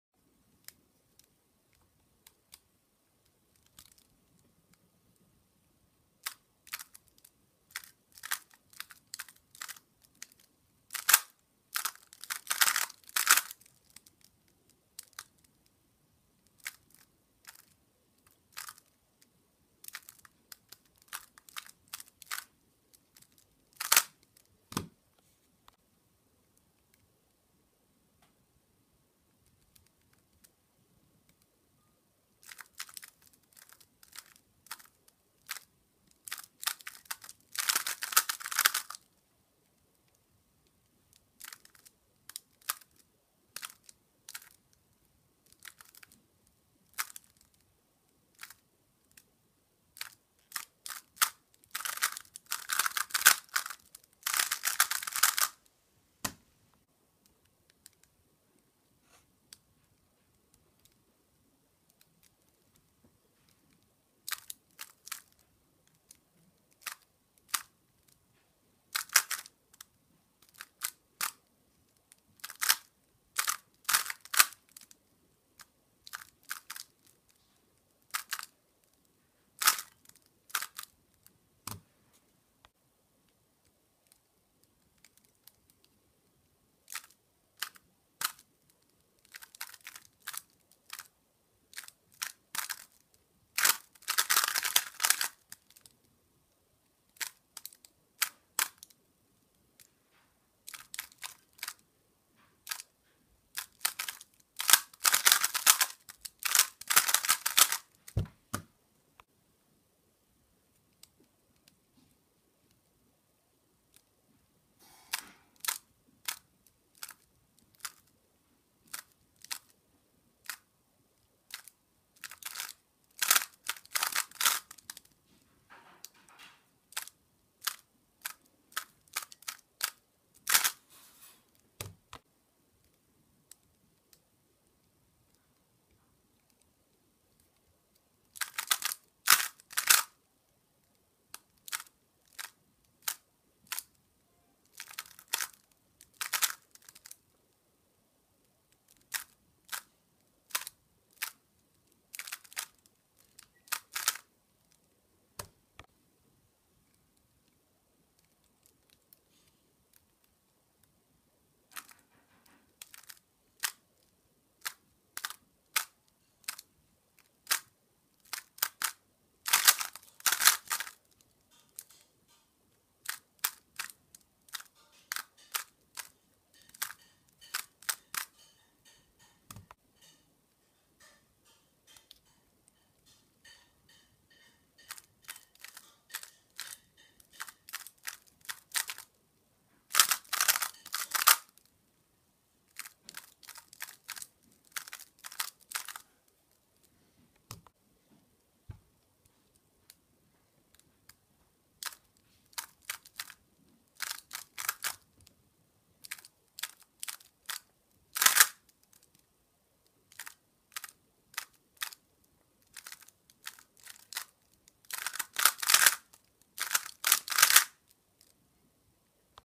Skewb Center on U, 5 Sledge Cases
Algs as they appear in the video;
1. r’ R’ z’ r B r’ R r y’ r’ R’ r
2.  r’ R’ r R r’ R’ r’ z’ r’ R’ r B
3. R r' R r' R' z' r' R r B' R
4. r' R' r R' B R B' R r' R' r R
5. r' R' r B R B' R' r' R r
6. R r R’ r’ z’ r’ R r z R r’ R’
7. r R' r' z' r' R' r z r R' r' R' r
8.  r' R r R r' z' r' R r z r R r'
9. r B R B’ r2’ R r R